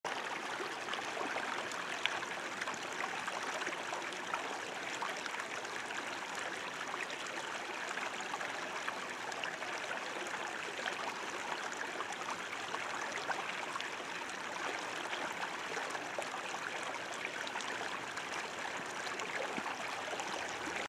Look at this spotted towhee's red eyes!
Spotted towhee (Pipilo maculatus)
January 2023
Pinnacles National Park, CA